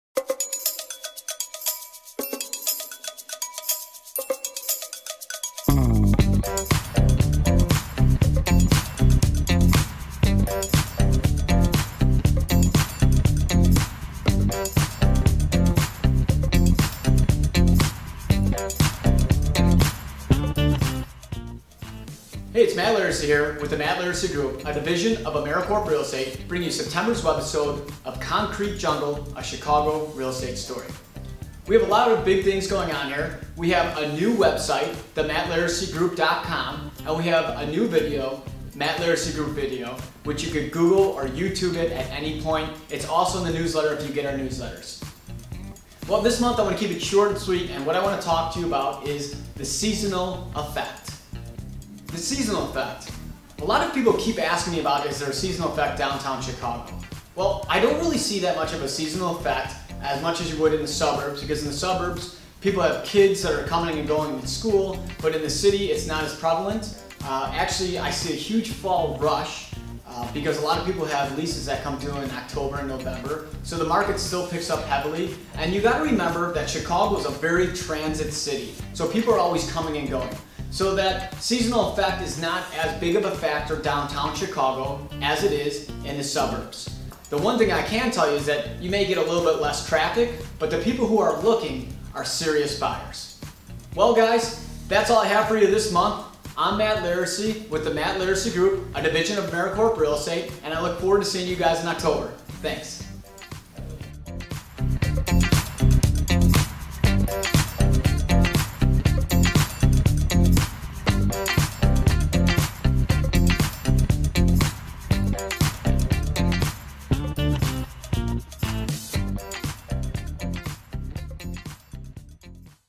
The Matt Laricy Group Concrete Jungle - S2 E7: The Seasonal Effect
This video is about Concrete Jungle - September Edition. Also have a look at our group video Lastly, Matt discusses if there is a "Seasonal Effect" in Downtown Chicago!